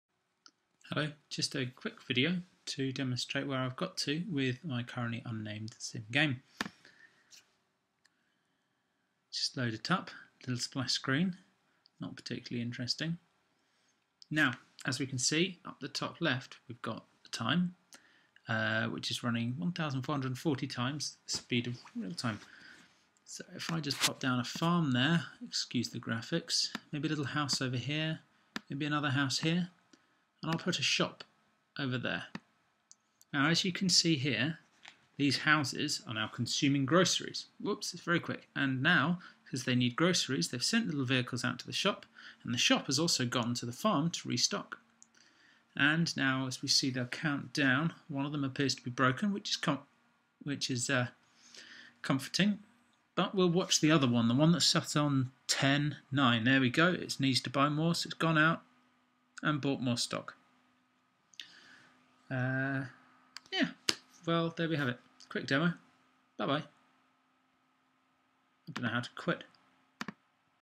SimGame1 Behaviours
This video demonstrates buildings which have behaviours attached to them performing a series of related activities. (Running out of food, then going to buy more.)

While I was on holiday I started work on a city simulator type game inspired by some of my favourites.

Concentrating on functionality before graphics (they are quite separate codebases) I have put together a Condition/Action based behavioural system where by world objects (buildings or vehicles) can have behaviours attached to them which will decide when to act, and then act.

This is a more applications oriented design than games oriented but I am trying it to see how it goes for making the environment extensible.

Next steps will be creating many more building types, and allowing the creation of zones will will build themselves based on environmental variables.